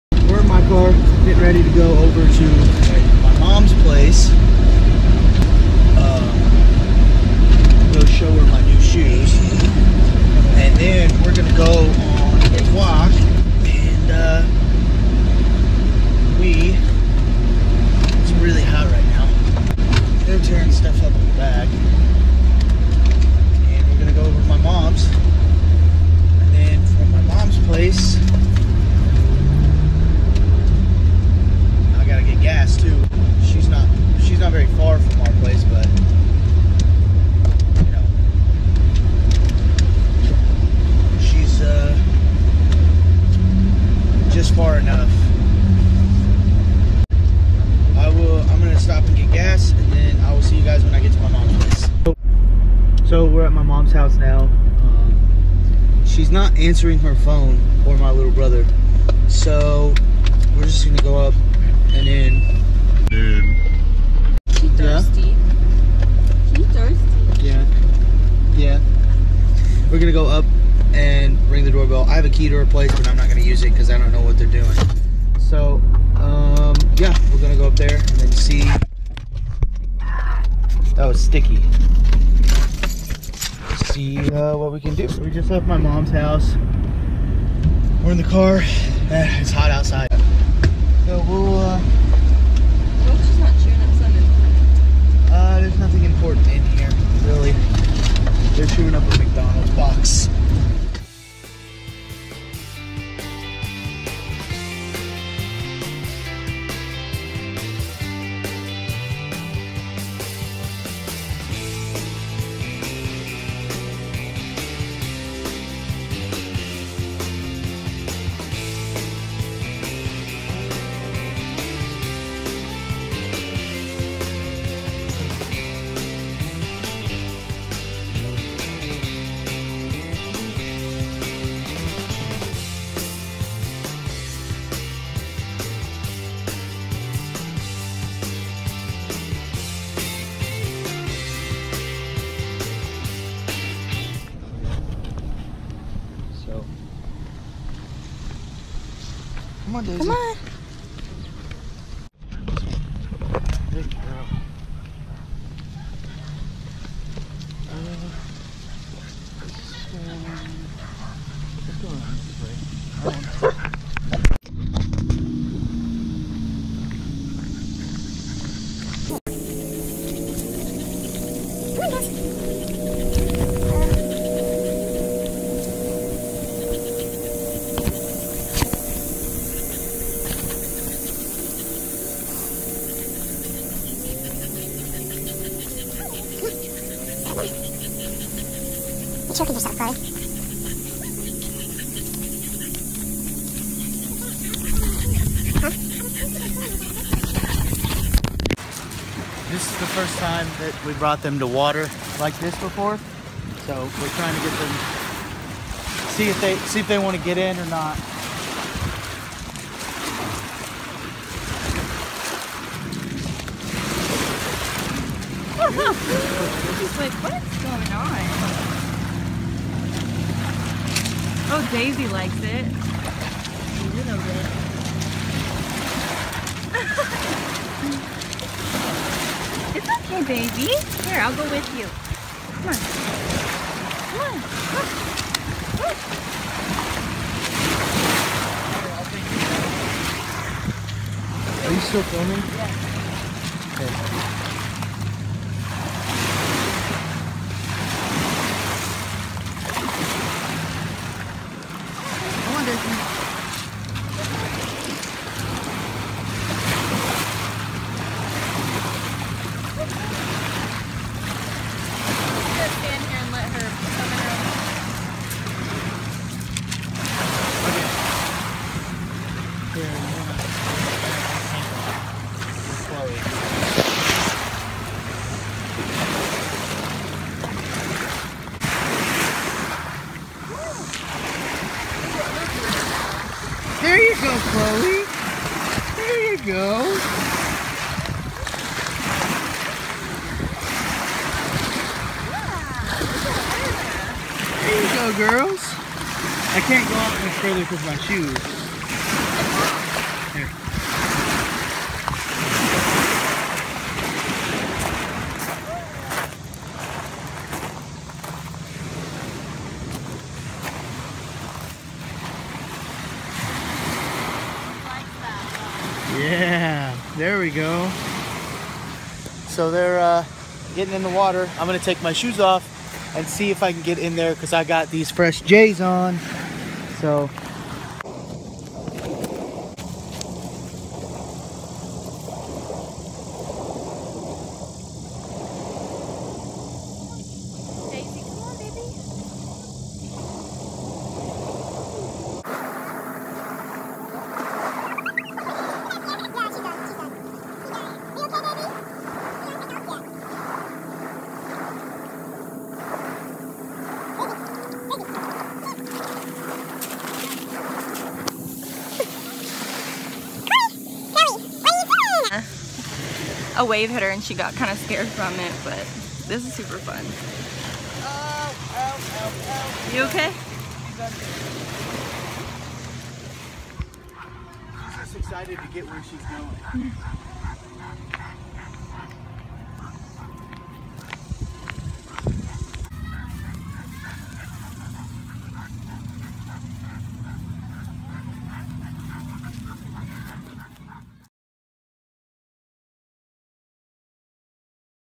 PUPPIES FIRST TIME IN NATURAL WATER!
🌊 This is the first time Daisy or Chloe have experienced the lake! We wanted to see how they reacted to it for when we move to Kaua'i. They weren't too crazy about the waves but it's okay! They'll get more used to it. Look forward to more vlogs and let us know where you want us to go! 🌊

see you in Kaua'i.